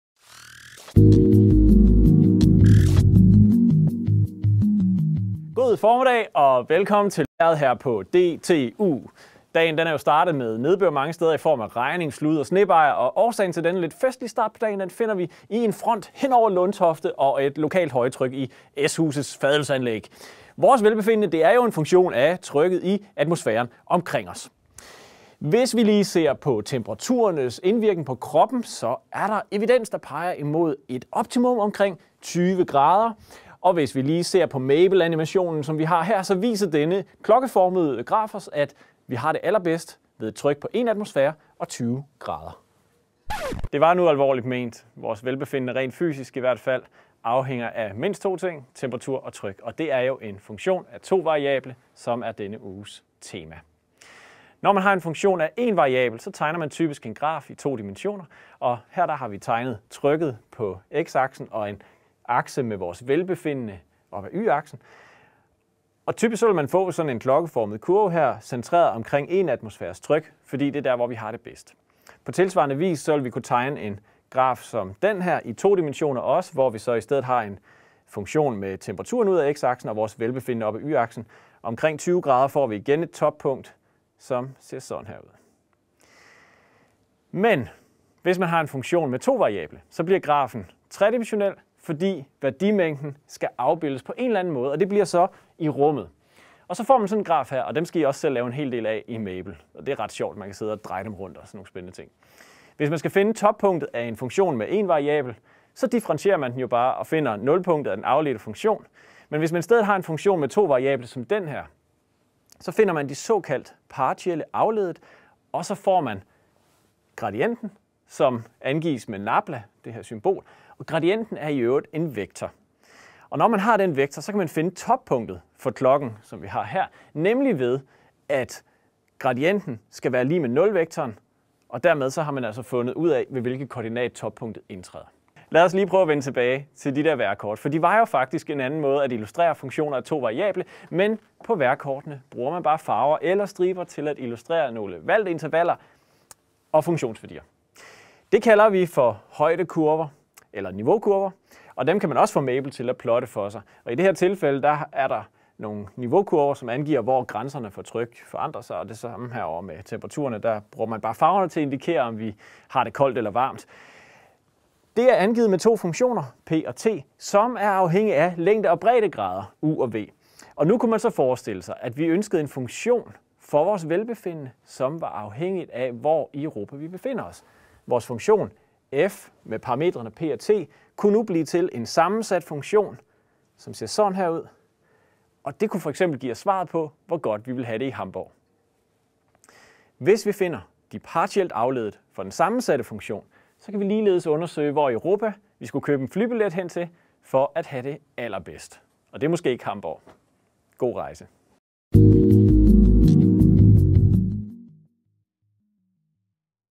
Uge 12: Funktioner af 2 variable
Hvad har funktioner af 2 variable med temperatur, tryk og vejrkort at gøre? Og hvor skal du mon rejse hen på juleferie for at ha' det allerbedst?